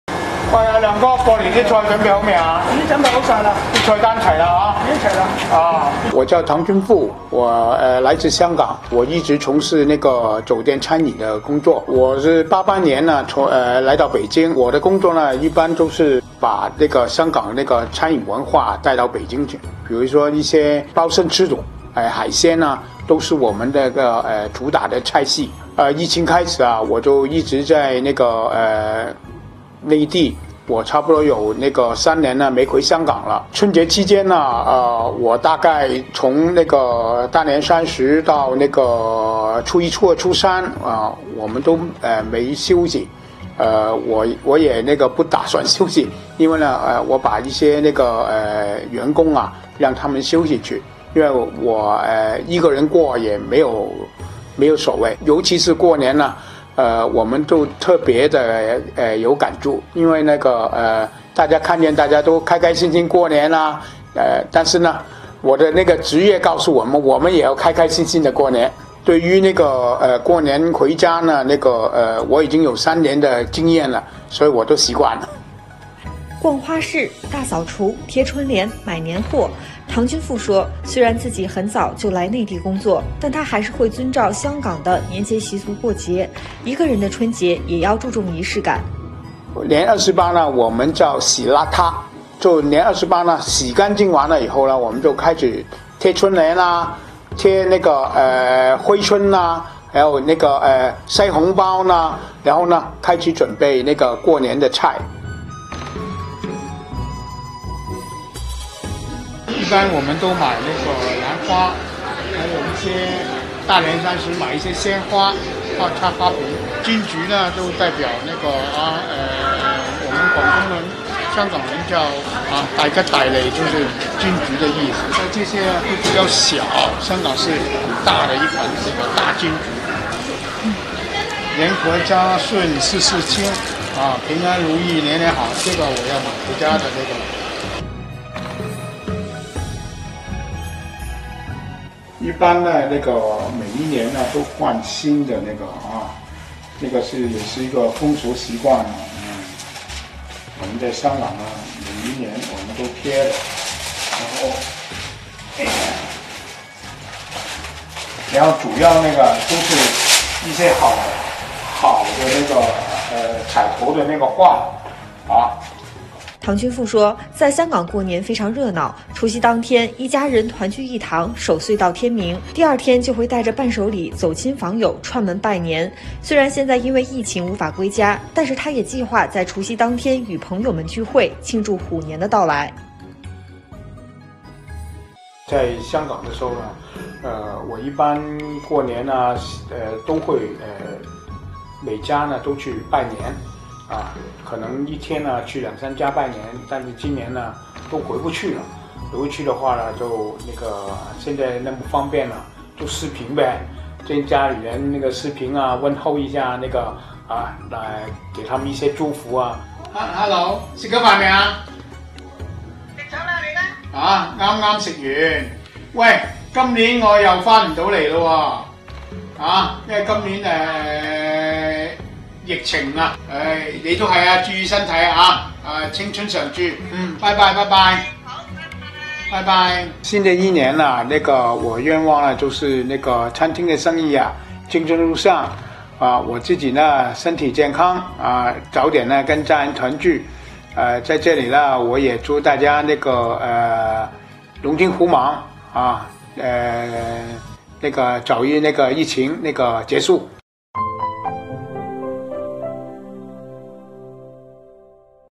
内地香港餐饮人的春节留守记
我叫唐君富，我来自香港，我一直从事酒店餐饮的工作。我是1988年来到北京，我的工作一般就是把香港的餐饮文化带到北京去，比如说一些鲍参翅肚、海鲜啊都是我们的主打的菜系。疫情开始我就一直在内地，我差不多有三年没回香港了。春节期间我大概从大年三十儿到初一、初二、初三，我们都没休息，我也不打算休息，因为我把一些员工让他们休息去，因为我一个人过也没有所谓。尤其是过年我们都特别地有感触，因为看见大家都开开心心的过年，但是我的职业告诉我们，我们也要开开心心的过年，对于（无法）过年回家我已经有三年的经验了，所以我都习惯了。
        逛花市、大扫除、贴春联、买年货，唐君富说，虽然自己很早就来内地工作，但他还是习惯遵照香港的习俗过年，一个人的春节也要注重仪式感。